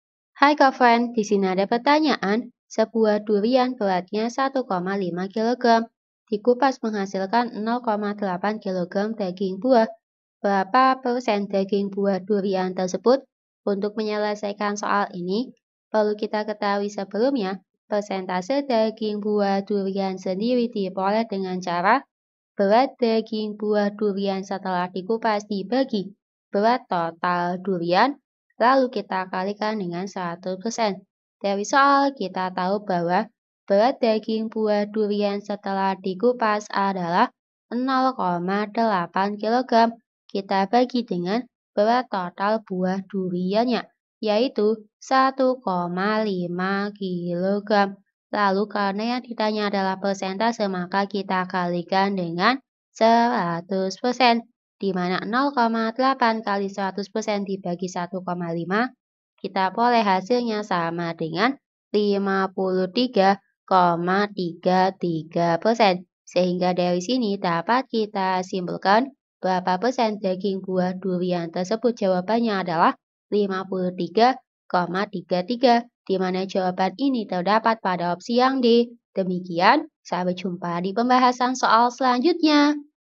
Sebuah durian beratnya 1,5 kg, dikupas menghasilkan 0,8 kg daging buah. Berapa % daging buah duri...
Sebuah durian beratnya 1,5 kg, dikupas menghasilkan 0,8 kg daging buah. Berapa % daging buah durian tersebut?

Topik atau Materi: Operasi pada Pecahan Desimal - BILANGAN BULAT DAN PECAHAN - BILANGAN - Matematika Kelas 7 SMP -

Sumber Soal: Student Feedback Apps Matematika Kelas 7, 2021 , N/A

00:00 : Bedah Soal
01:00 : Konsep, rumus dan pengertian Operasi pada Pecahan Desimal
01:18 : Langkah penyelesaian soal
00:23 : Jawaban akhir
00:53 : Penutup